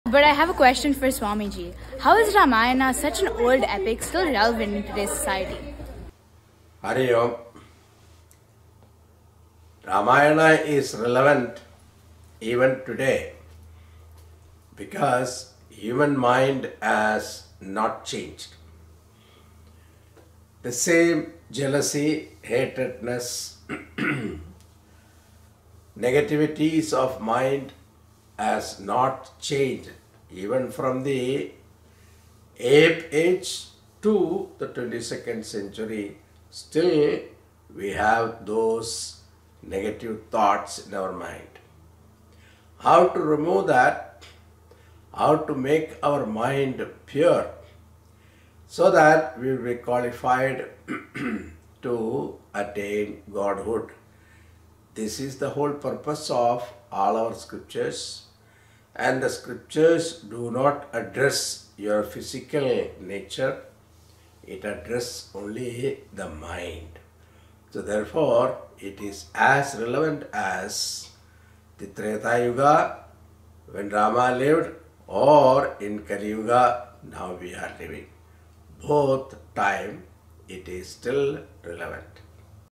Is Ramayana still relevant today?
How is Ramayana an old epic still relevant in these times? Q&A with Swami Shantananda ji